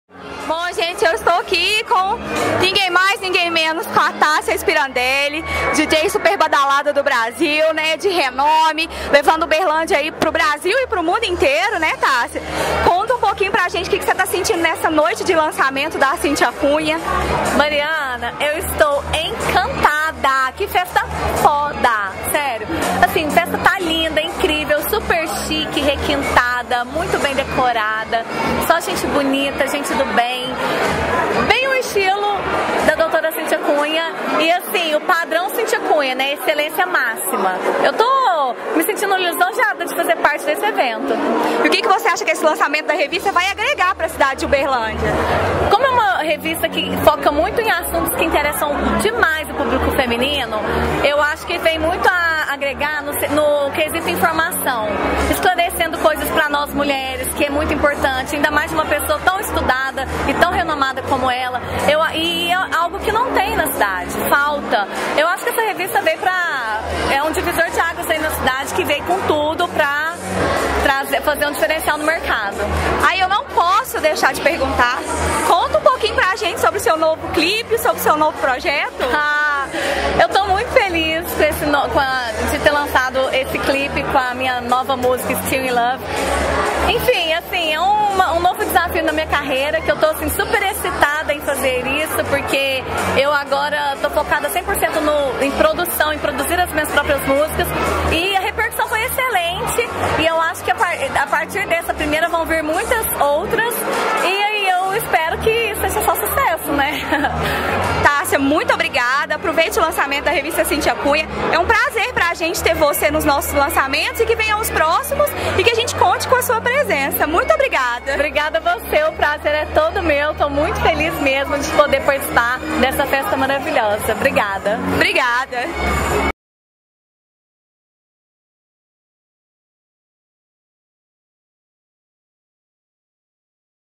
Dj Thascya Spirandelli no lançamento Cintia Cunha
Ela é a melhor Dj Mulher do Brasil, linda, encantadora e mãe da Sol. Confiram o que a Dj Thascya Spirandelli falou sobre o lançamento da Revista Cintia Cunha!